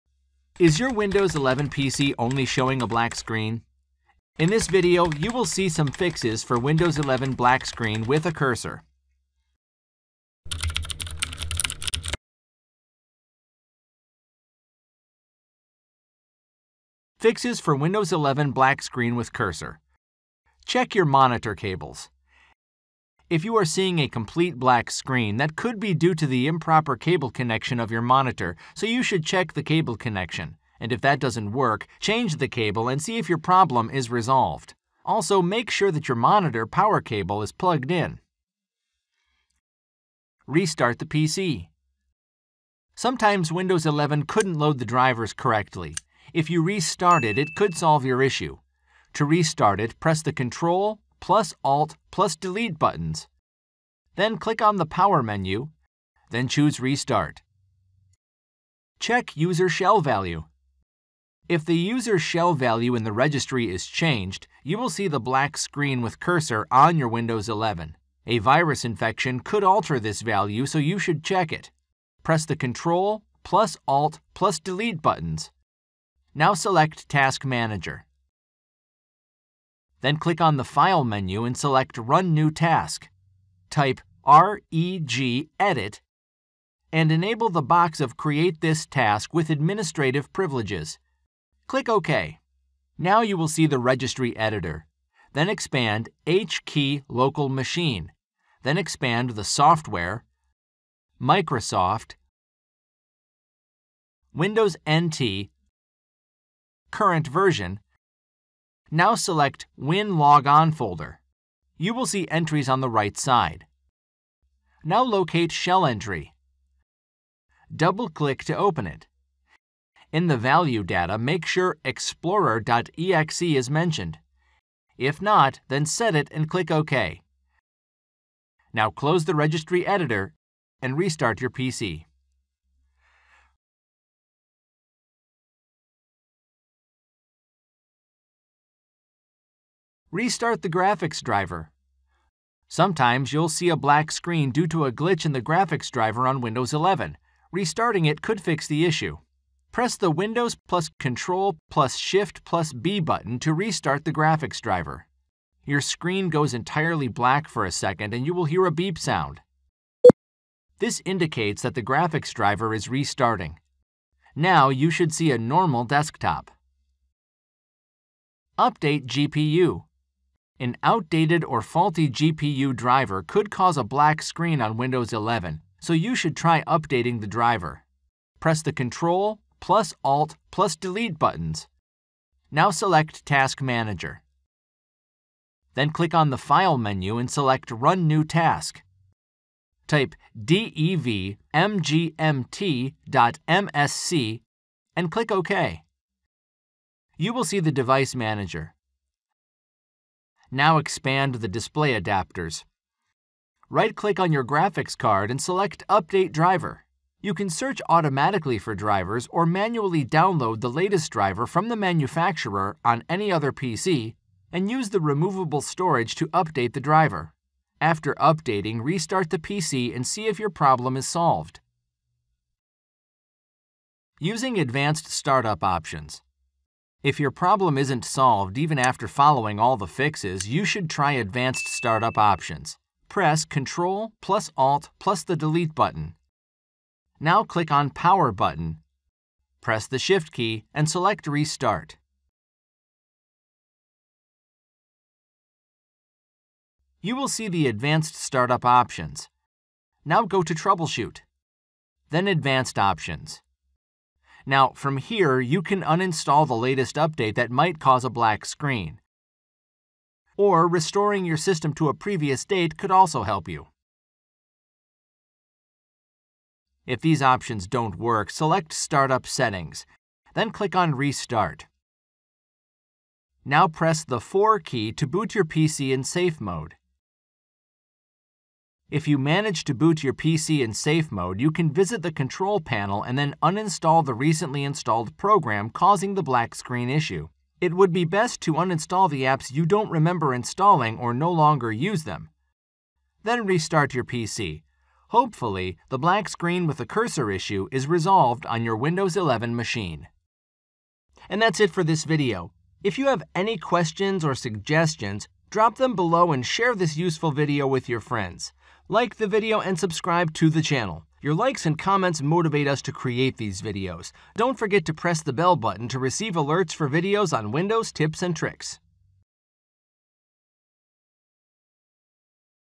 How to Fix Black Screen with Cursor on Windows 11?
How to fix black screen with the cursor on Windows 11? Is your Windows 11 PC is only showing a black screen? In this video, you will see the fixes for Windows 11 black screen with cursor. A black screen usually occurs when the shell value in the registry is changed by you mistakenly or by a virus. The video includes how to check the shell value and other fixes for windows 11 black screen of death.

This is a troubleshooter video that you can use to fix Windows 11 bugs. This Windows 11 error can be fixed using several methods. We have included all possible fixes to these Windows 11 problems.

0:00 Intro
0:20 Check your Monitor Cables
0:40 Restart the PC
0:58 Check User Shell Value
2:19 Restart the Graphics Driver
2:48 Update GPU
3:44 Use Advanced Startup Options